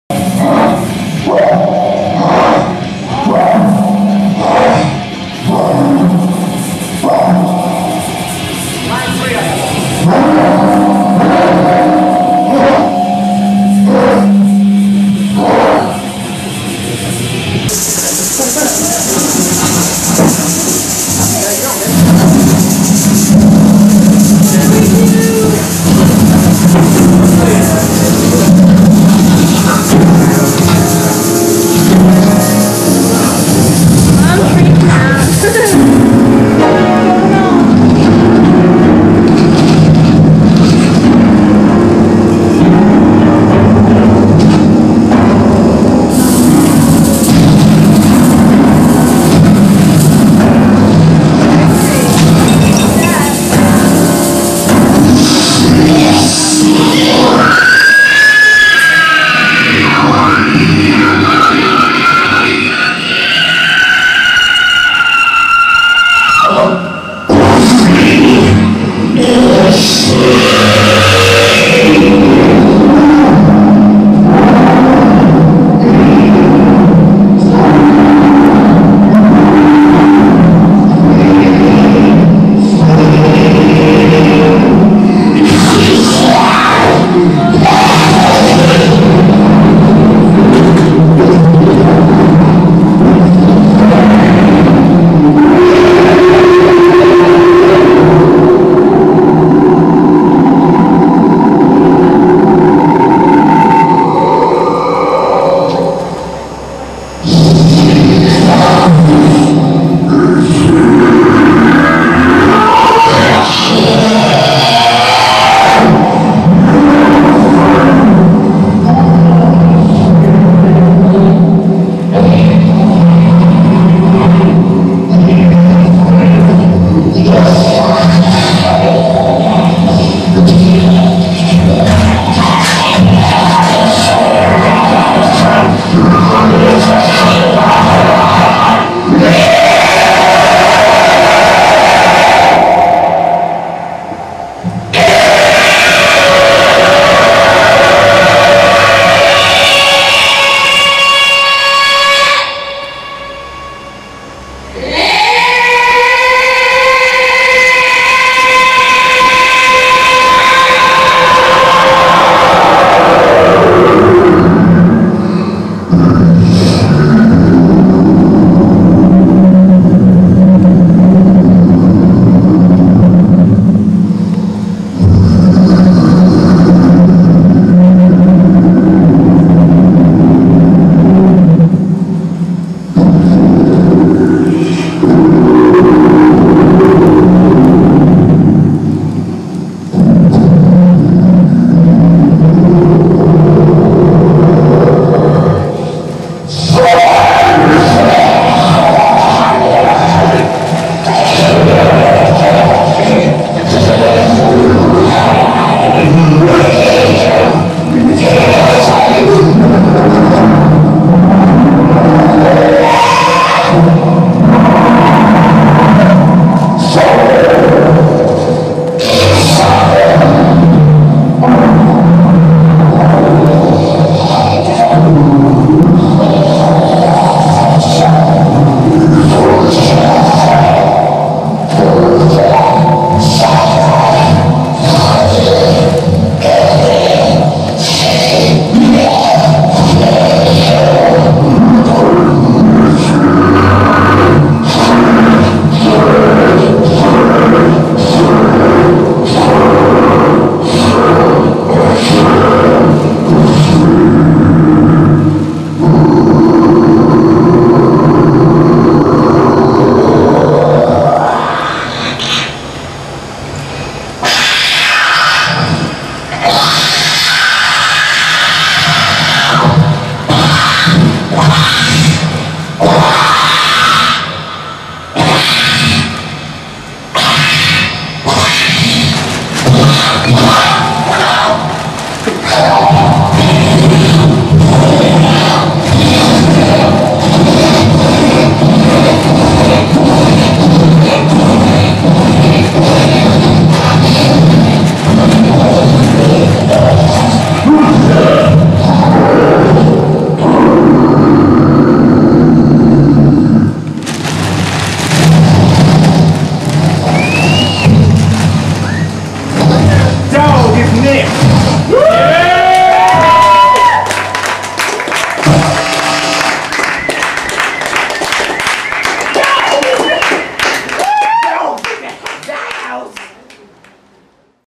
ANDORKAPPEN live @ Il Corral 09/07/07
I'm wearing some sort of metallic plastic bag on my head for this one. Brutal drunken vokill noiz aktion.